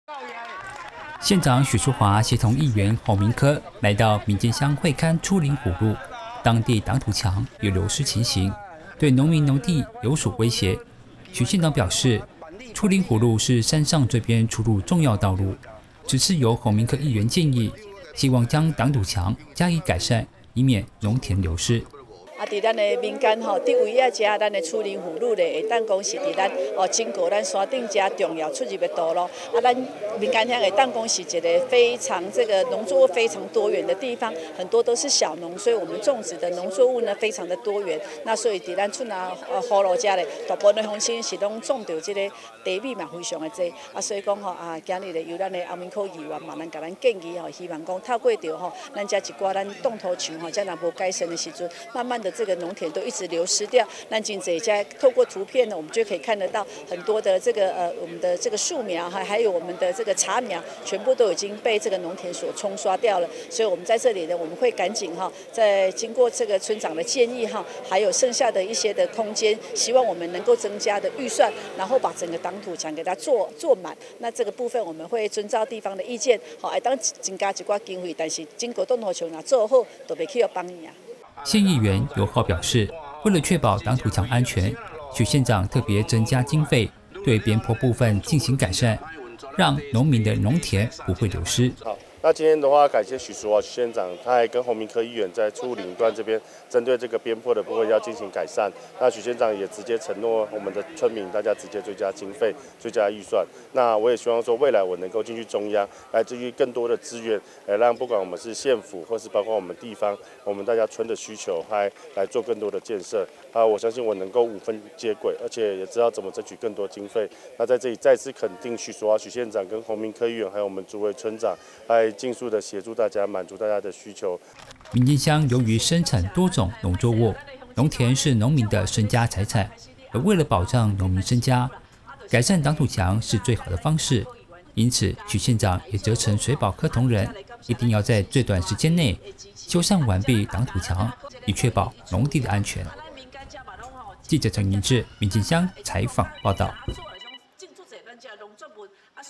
縣府會勘出林虎路擋土牆 協助改善防農田流失｜南投縣政新聞 2023.11.02
縣長許淑華在農業處水土保持科陪同下，會同縣議員洪明科來到名間鄉會勘出林虎路擋土牆工程，許縣長表示：為了確保農田不流失，縣府將盡速將擋土牆修護，以保障農民身家財產。